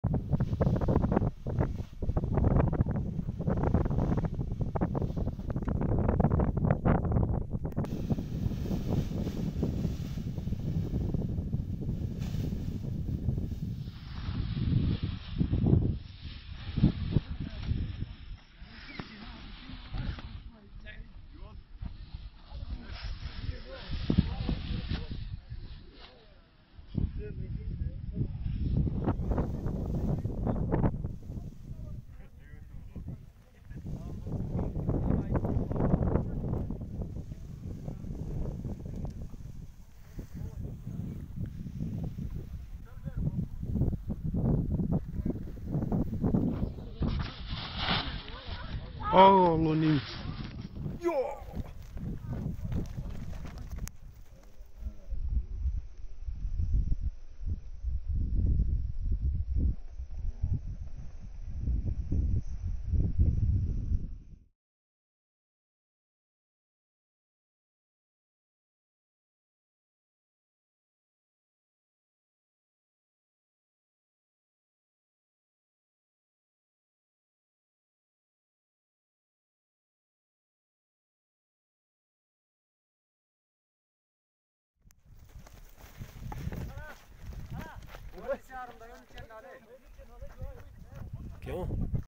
Alex Yaschenko Accident Hunza
Alex's accident.  This video was made up of parts of two separate independent videos from two different spectators.   Neither got the whole flight on video but luckily, the parts almost perfectly interacted so was able join parts each to get pretty much the entire flight on one video